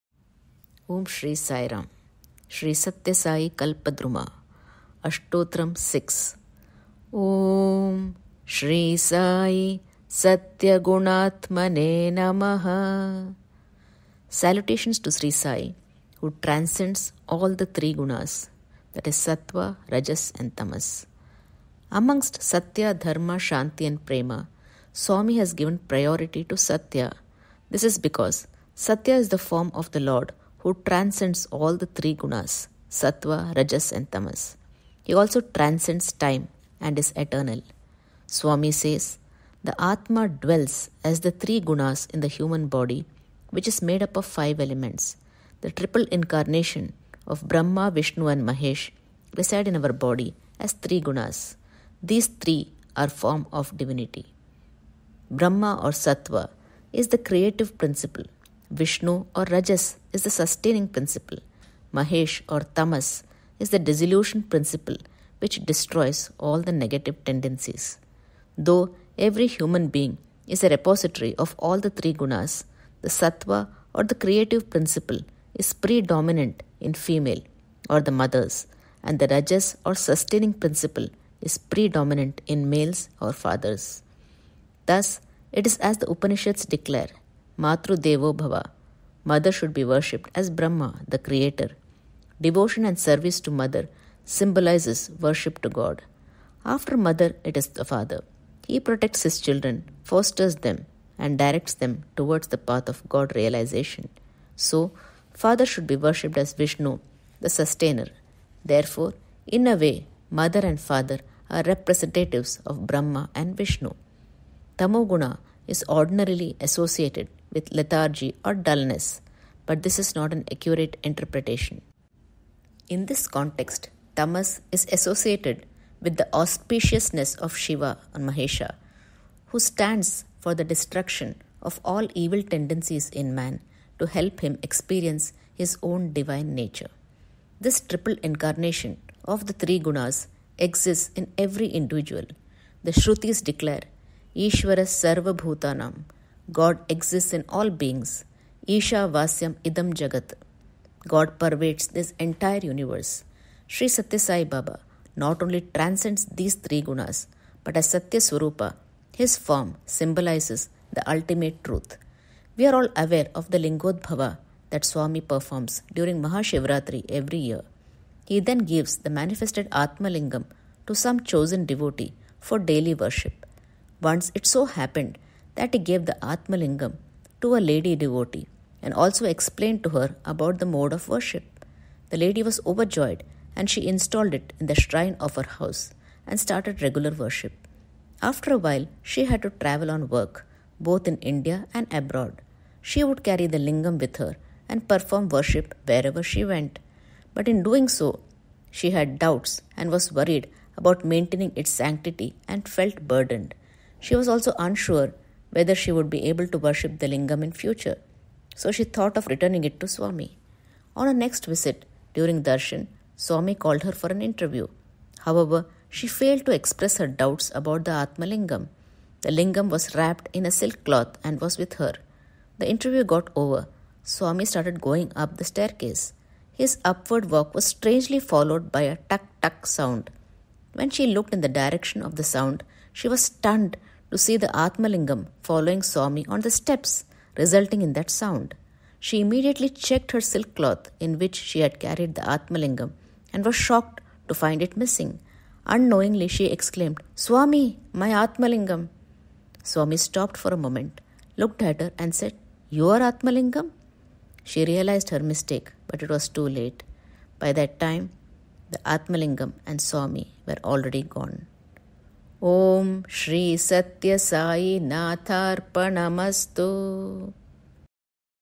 Sri Sathya Sai Kalpadruma- Ashtotram 06
Sai's glory through His ashtotram. From a gem of 108 names of Bhagavan this is the sixth name describing Sai Mahima
Om Sri Sai Sathya Gunaatmane Namah
Salutation to Sri Sai one who transcends all the 3 gunas (Sathwa, Rajas and Tamas)